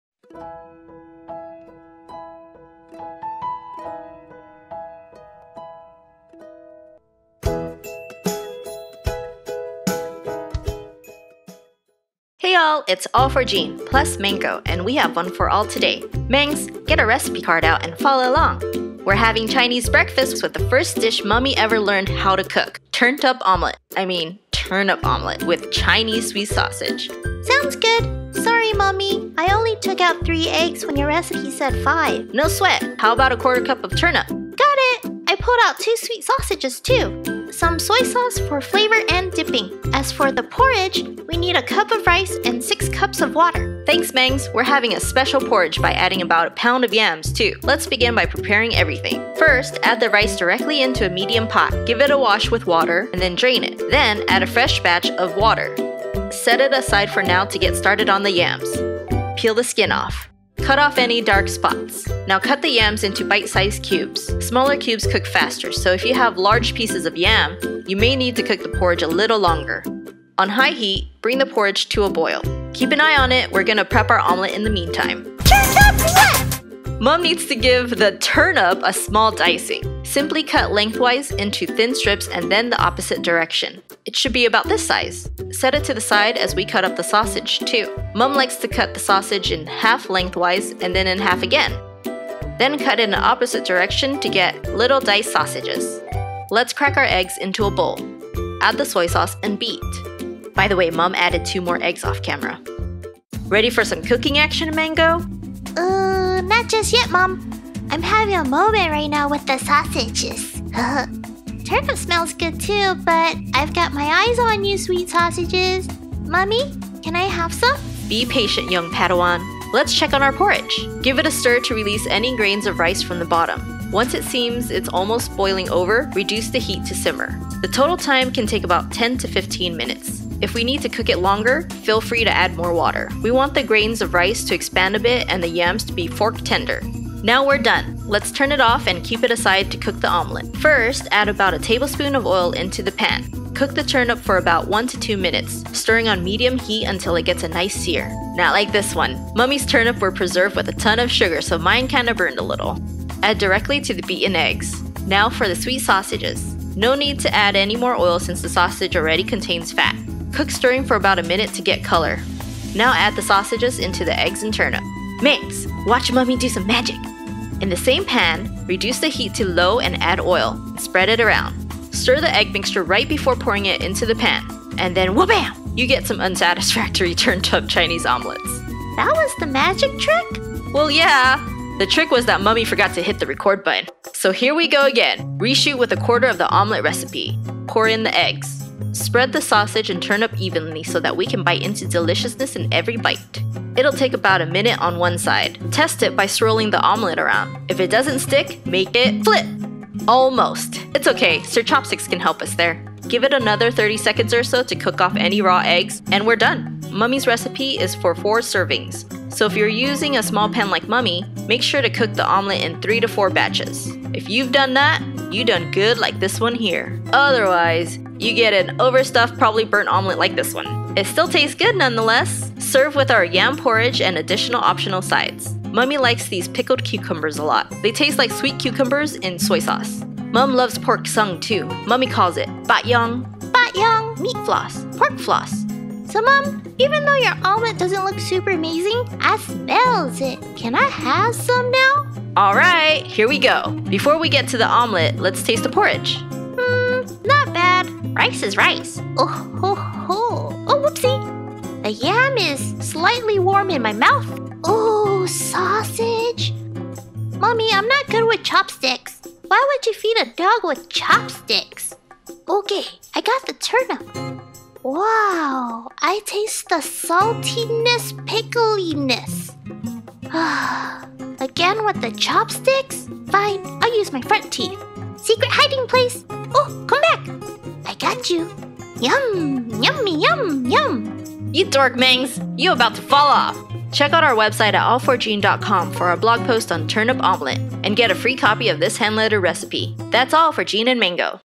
Turnip Omelette | a4J Recipe #7
We hope you can try this recipe out. It's a classic Chinese breakfast pairing.

DIY!
(Jean designs and hand writes all the recipe titles you see in the thumbnail, ALL ORIGINAL)

Follow Mang0 the Spacedog!

Follow Jean!

ABOUT US:
We rebranded!!! Mang0, Jean and Andre (gOjeandre) is still the same ol' all4jean channel & content.